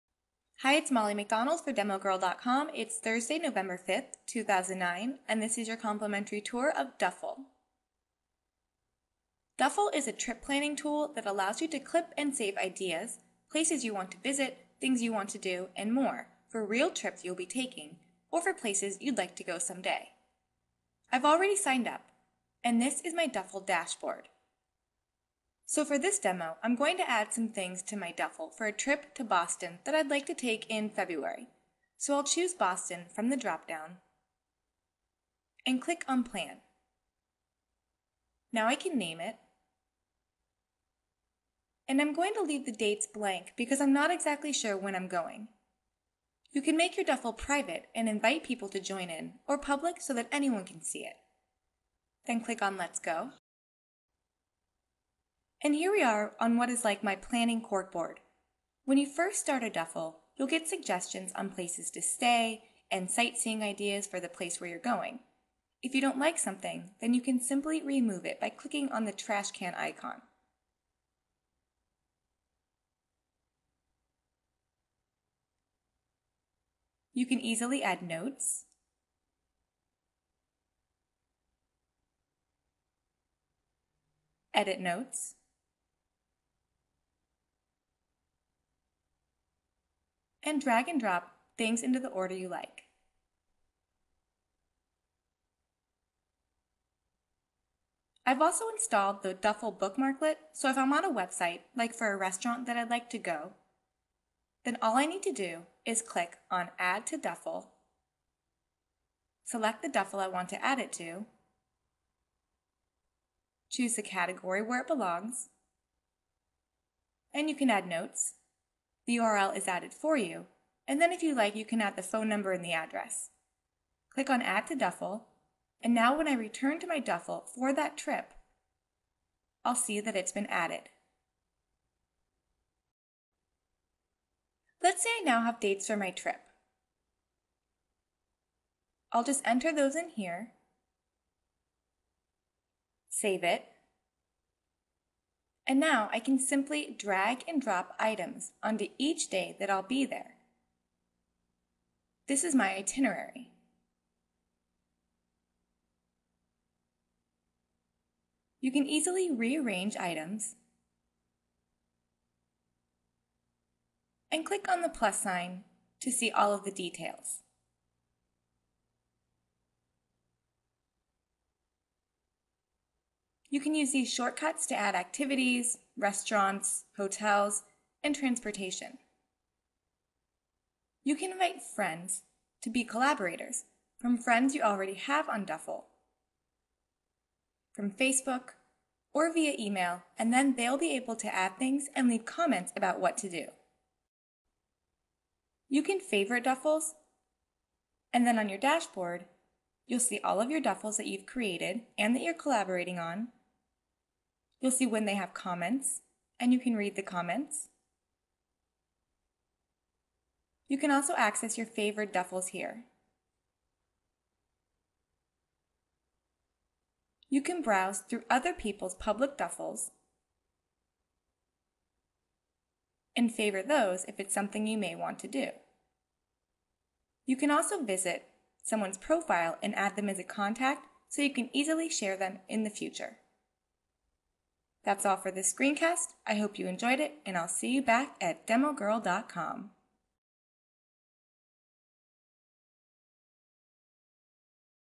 Screencast Tour of DuffeUp.com
Duffel is a tool that helps you find and save ideas for trips you're planning and for places you hope to go one day.